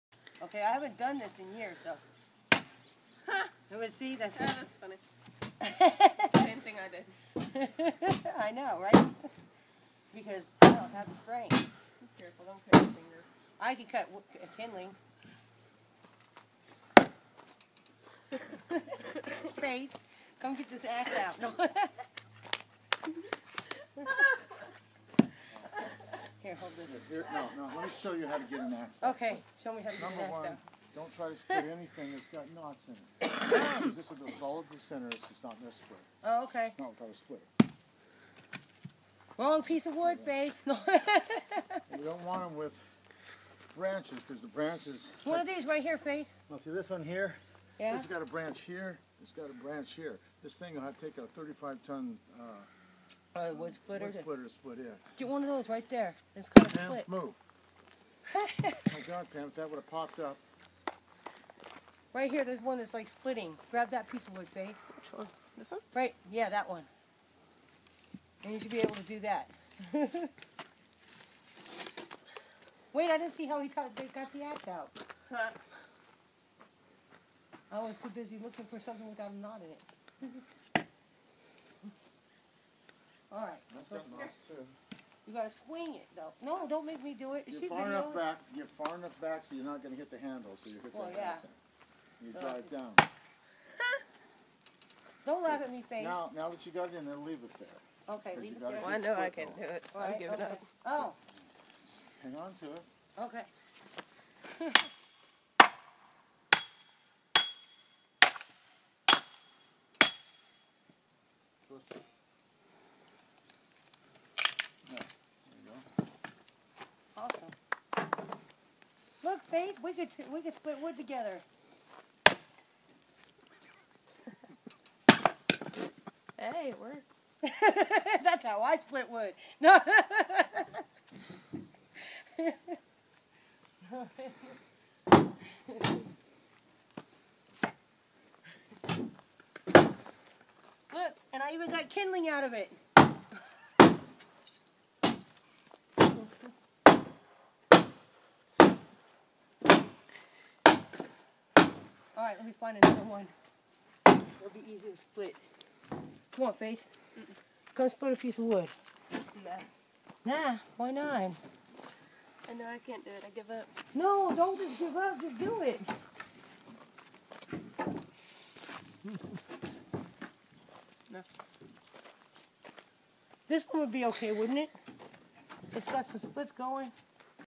wood splitting lesson
Daughter and granddaughter learning to split wood.